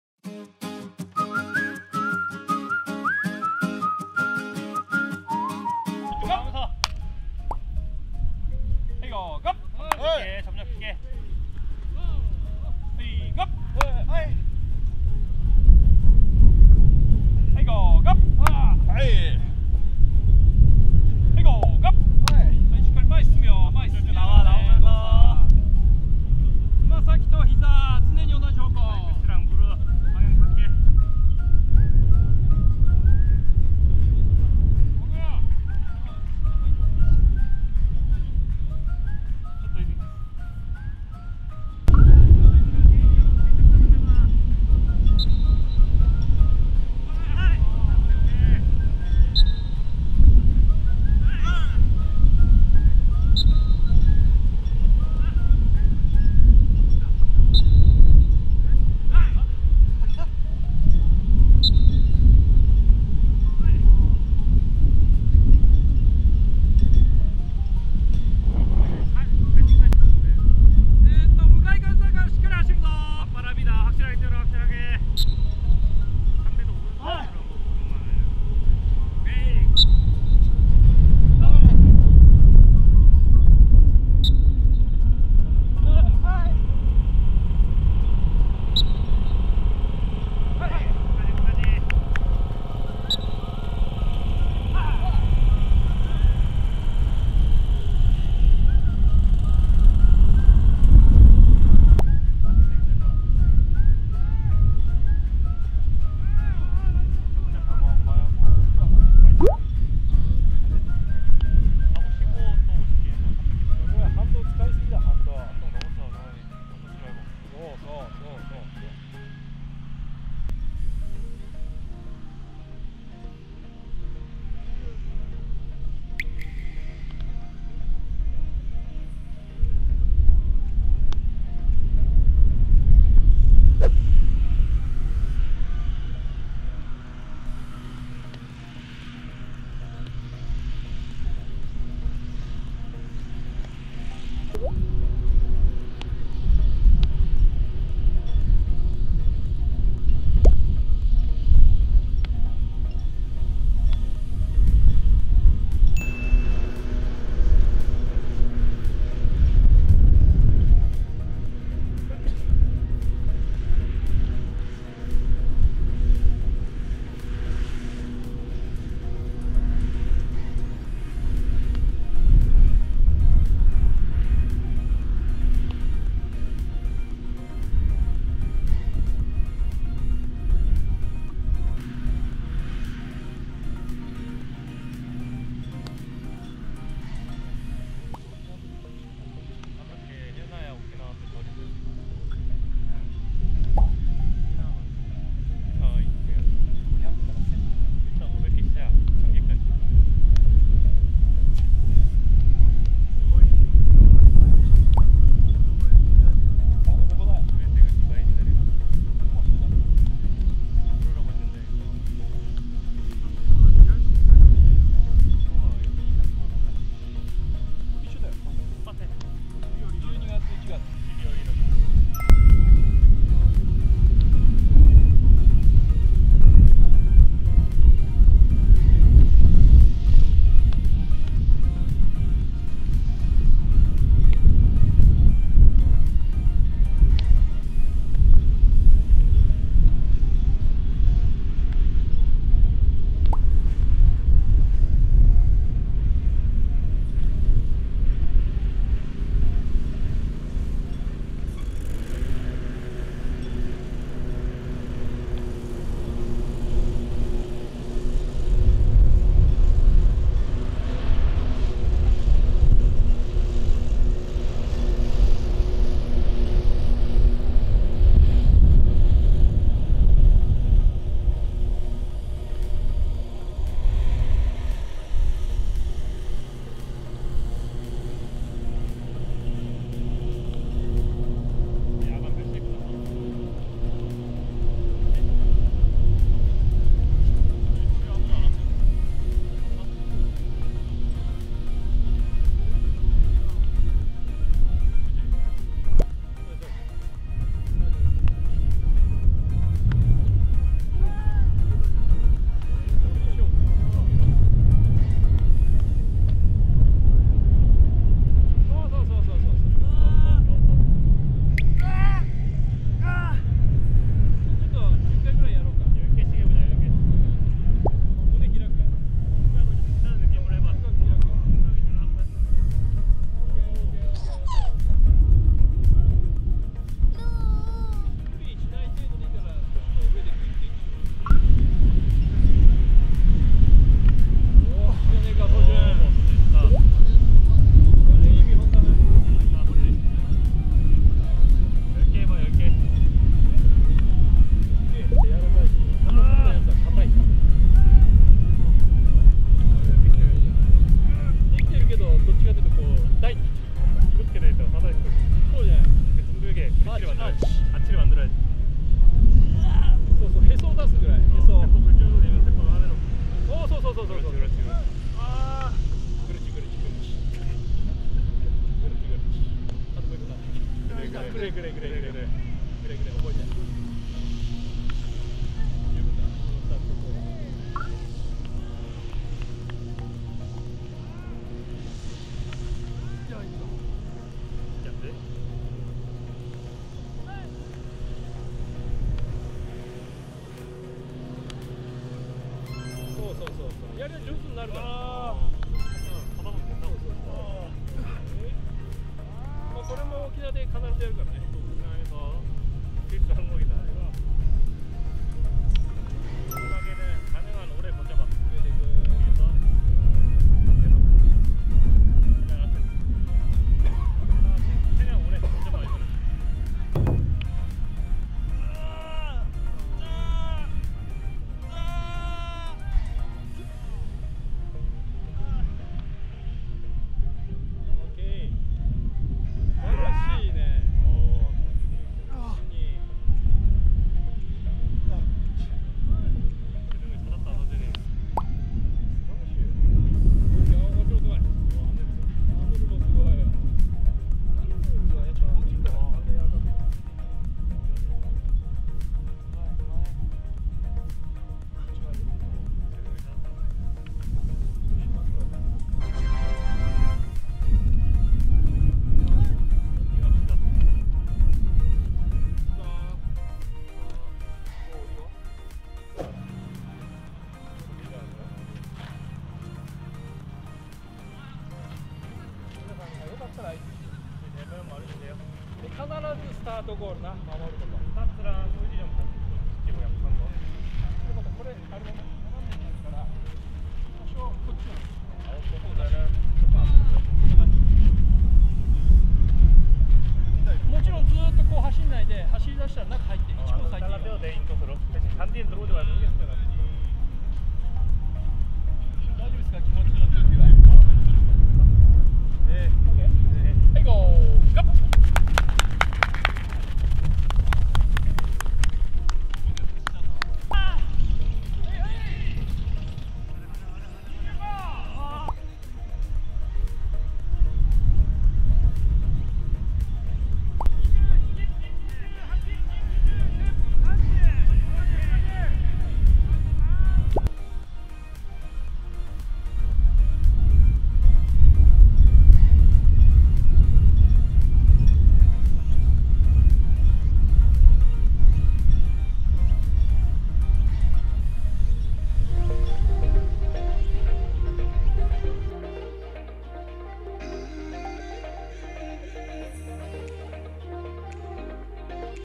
새로운 재능을 찾은 코어훈련
비 내린 다음날 야외에서 진행하는 투수조 훈련
화창한 미야자키 하늘 아래서 코어훈련